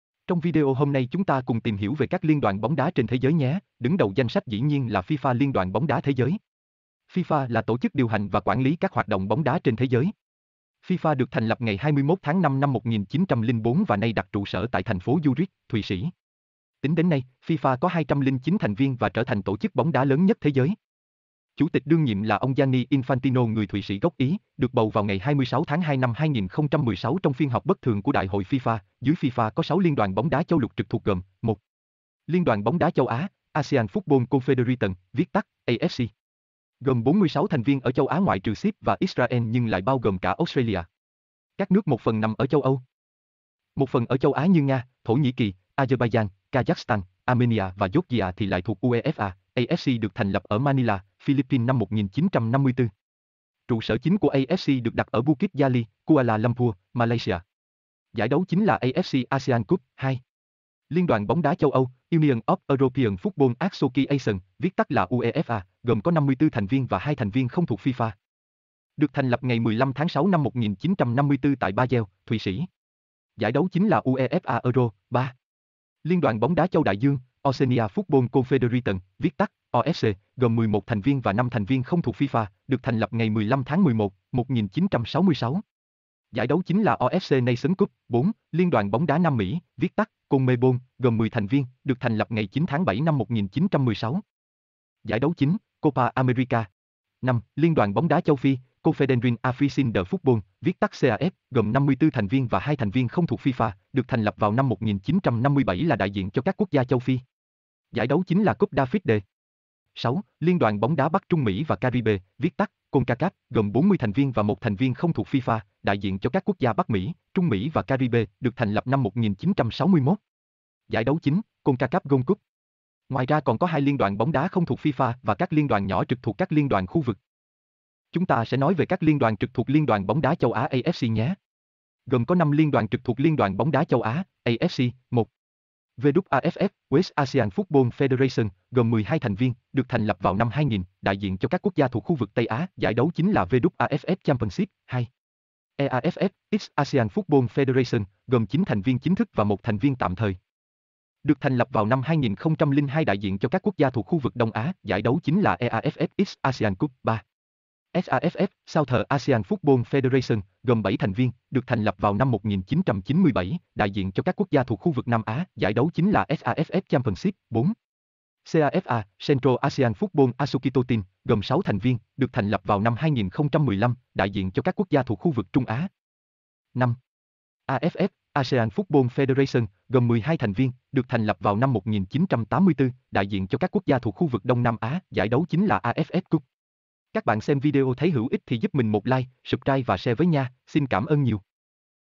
BẠN ĐÃ BIẾT ĐƯỢC BAO NHIÊU "LIÊN ĐOÀN BÓNG ĐÁ THẾ GIỚI" ? FIFA. AFC, UEFA, WAFF, CONCAF,....
BẠN LÀ FAN BÓNG ĐÁ THỰC THỤ, VẬY BẠN CÓ BIẾT CÁC LIÊN ĐOÀN BÓNG ĐÁ THẾ GIỚI KHÔNG? XEM VIDEO ĐỂ TÌM CÂU TRẢ LỜI CHO MÌNH NHÉ